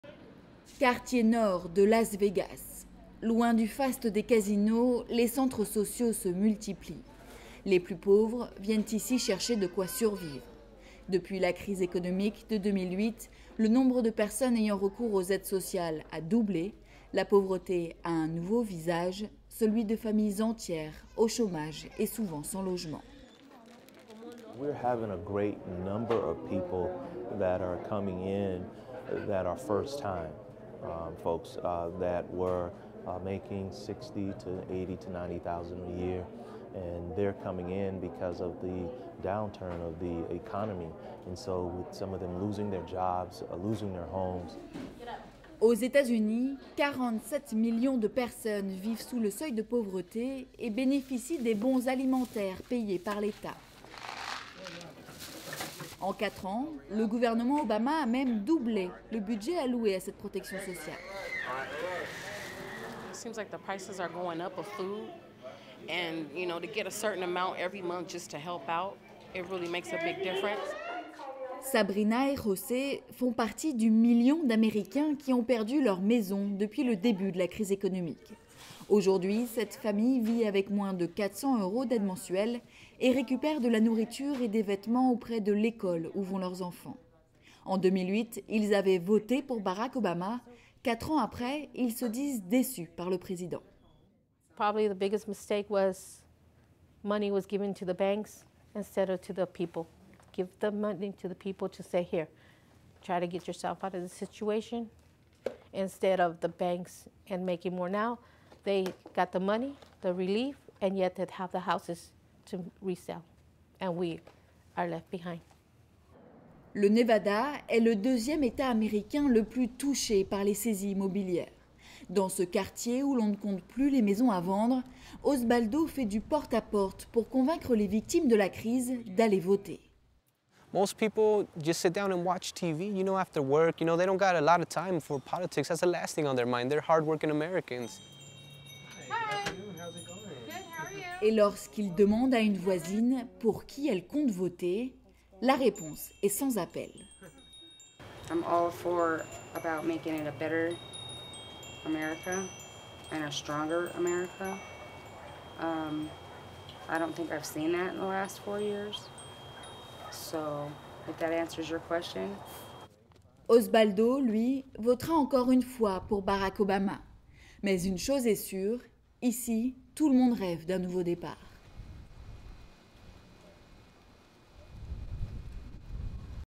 Les plus démunis pourraient tourner le dos à Barack Obama
Aux Etats-Unis, 46 millions de personnes vivent sous le seuil de pauvreté... Un nombre en augmentation de 27% depuis le début de la crise économique en 2007. La hausse du chômage et la crise des subprimes ont plongé des familles entières de la classe moyenne dans la pauvreté. Alors ceux qui avaient soutenu Barack Obama il y a 4 ans, s'interrogent... Certains pourraient lui tourner le dos lors du scrutin du 6 novembre..
Exemple dans l'un des swing state du Grand Ouest, le Nevada, l'un des Etats américains les plus touchés par les saisies immobilières et qui compte le plus au taux de chômage du pays. Les déçus d'Obama peuvent-ils faire pencher la balance pour Mitt Romney ? Reportage dans la banlieue de Las Vegas.